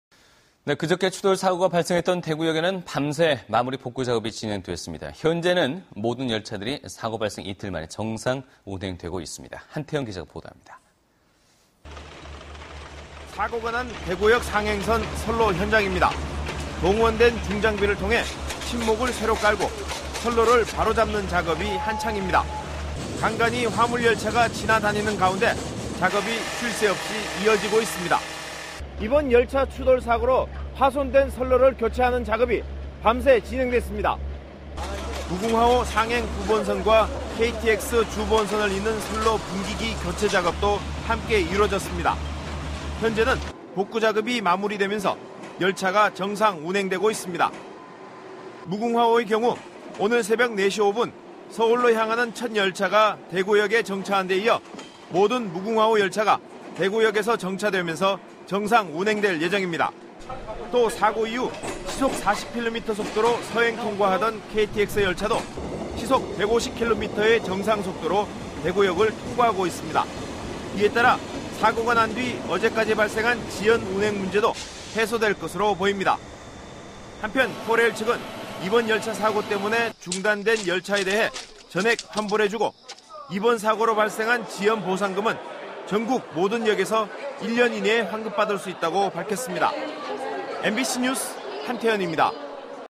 [대구MBC뉴스] 대구역 복구완료 정상 운행 시작
내용: 대구역 정상 운행 시작 - 한태연 기자